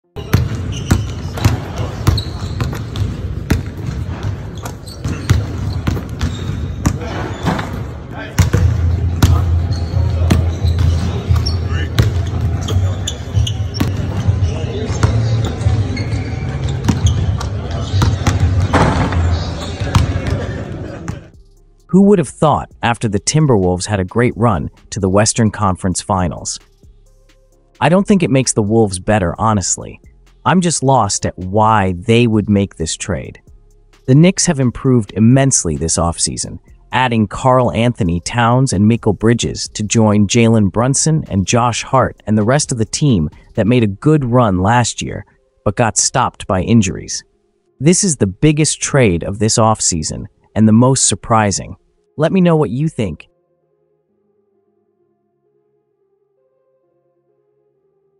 Karl Anthony Towns Has First Practice With New York Knicks Since Getting Traded | KAT Knicks
Kat has his first practice with the New York Knicks since joining them in a trade with Julius Randle last week